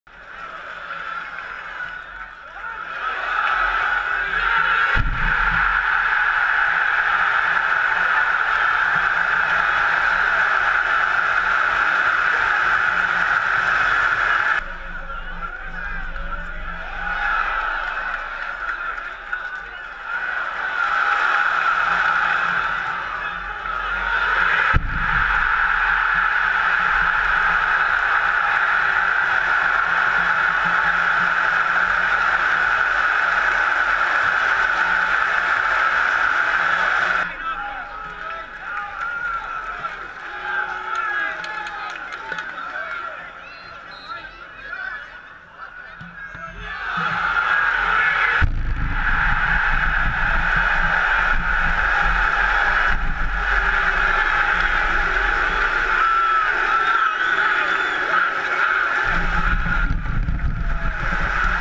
GO PRO GOALS: TOM EAVES HAT-TRICK
The GFC Goal Cam captures Tom Eaves netting a hat-trick against Southend United in Sky Bet League 1.